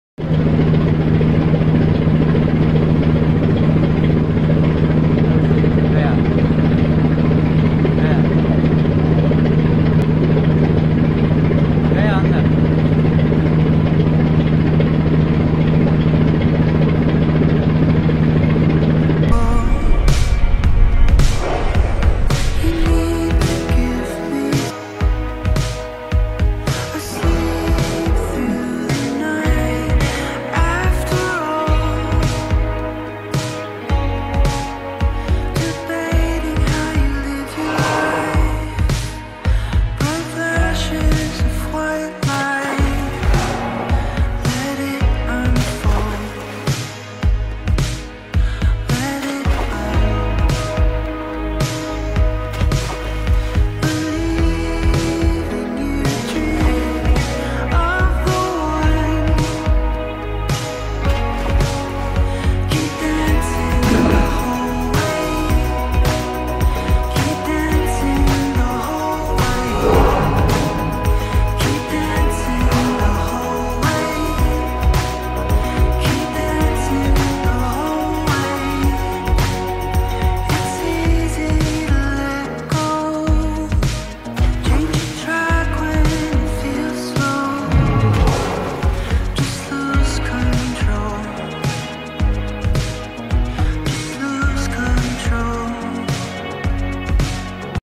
we spotting amazing whales
We spotting amazing whales  .its amazing experience  .it is the beauty  of nature that cannot described in words . it seems as showing us the way.